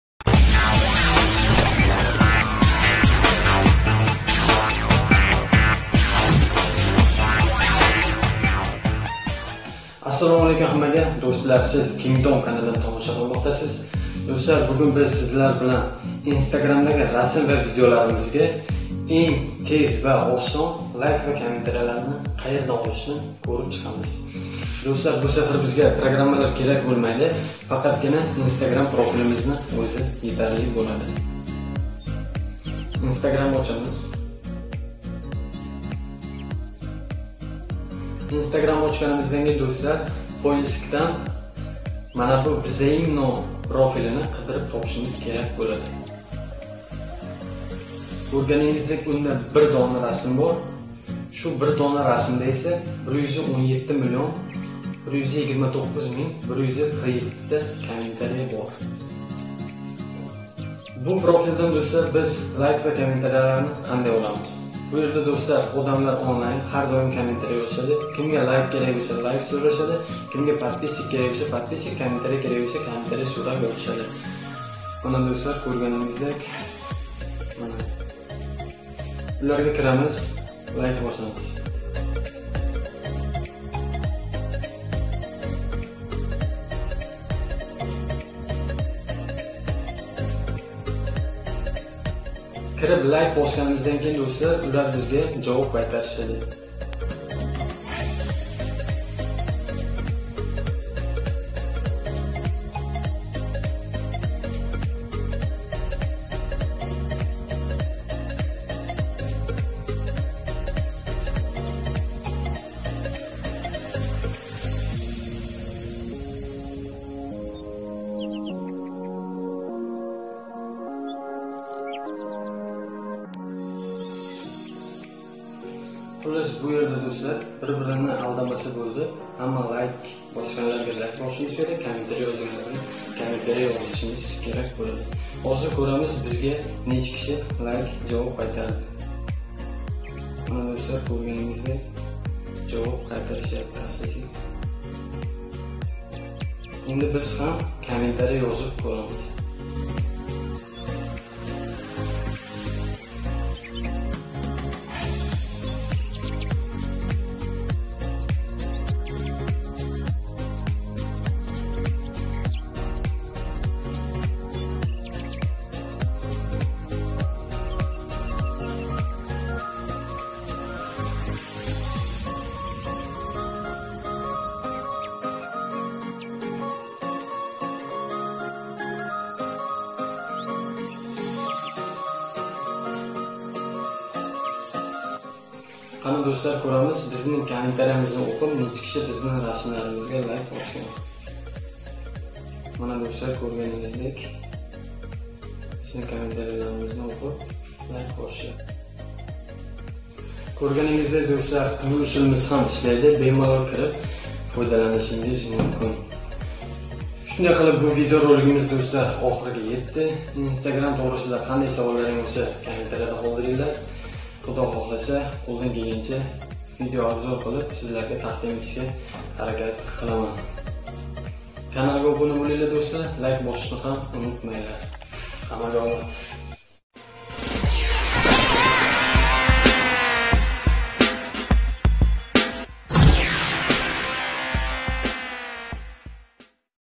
INSTAGRAMDA TEZ VA OSON LIKE KOMMENTARIYA YIG'ISH | INSTAGRAM SIRLARI 2020 | jafar off
Hammaga SALOM! Dustlar ushbu videoda siz instagramdagi postlar uchun tez va oson like kommentariya yig'ishni urganib olishingiz mumkin ...

🔕Videoga like bosib kommentariya yozishni unutmang